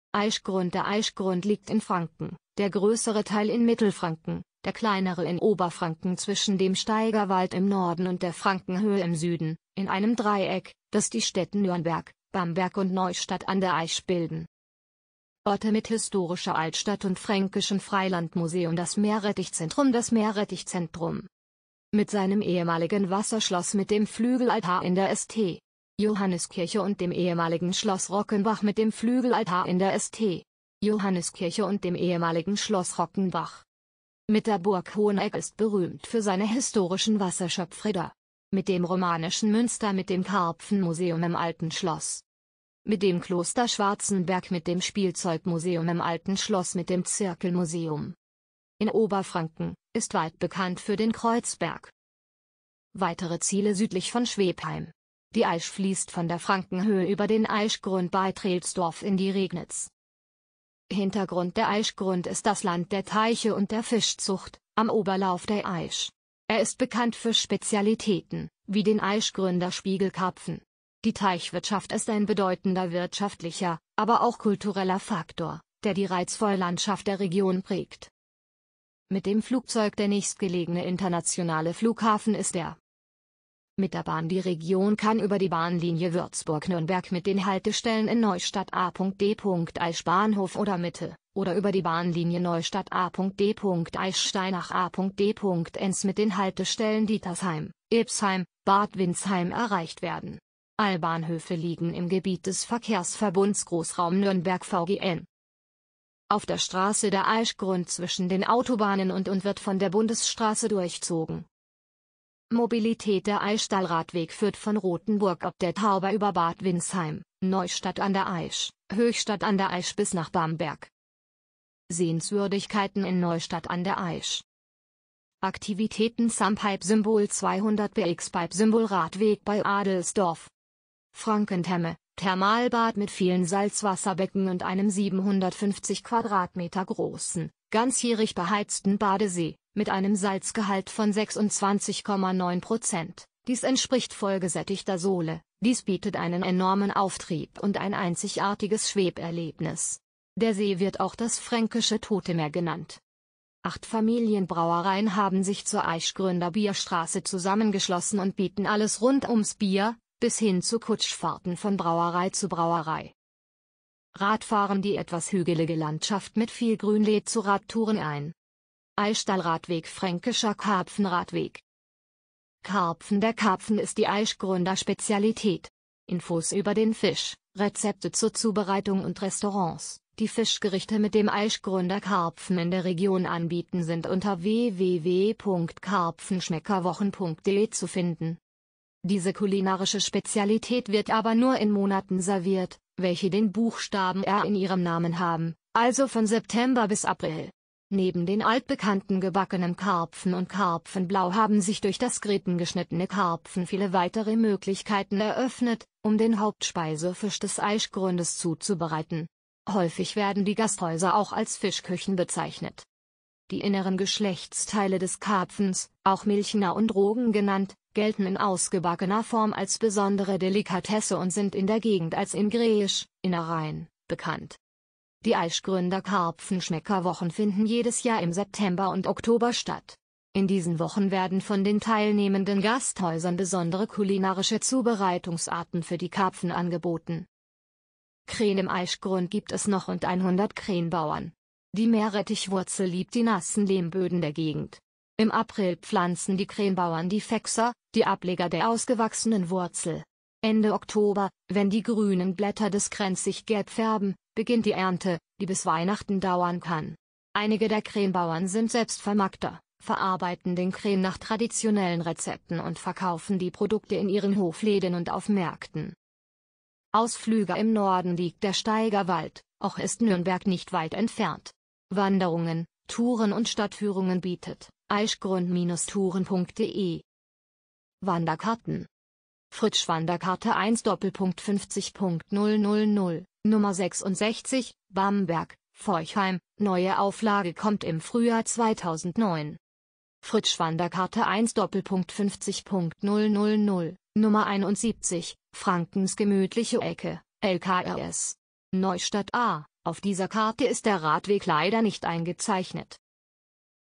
Aischgrund | Travel Tipps | Reiseführer Deutsch
Der Aischgrund liegt in Franken, der größere Teil in Mittelfranken, der kleinere in Oberfranken zwischen dem Steigerwald im Norden und der Frankenhöhe im Süden, in einem Dreieck, das die Städte Nürnberg, Bamberg und Neustadt a. d. Aisch bilden.

 * - mit historischer Altstadt und Fränkischem Freilandmuseum
 * - das Meerrettichzentrum
 * - das Meerrettichzentrum

 * - mit seinem ehemaligen Wasserschloss
 * - mit dem Flügelaltar in der St.-Johannis-Kirche und dem ehemaligen Schloss Rockenbach

 * - mit der Burg Hoheneck
 * - ist berühmt für seine historischen Wasserschöpfräder.
 * - mit dem romanischen Münster
 * - mit dem Karpfenmuseum im Alten Schloss

 * - mit dem Kloster Schwarzenberg
 * - mit dem Spielzeugmuseum im Alten Schloss
 * - mit dem Zirkelmuseum

In Oberfranken:
 * - ist weit bekannt für den Kreuzberg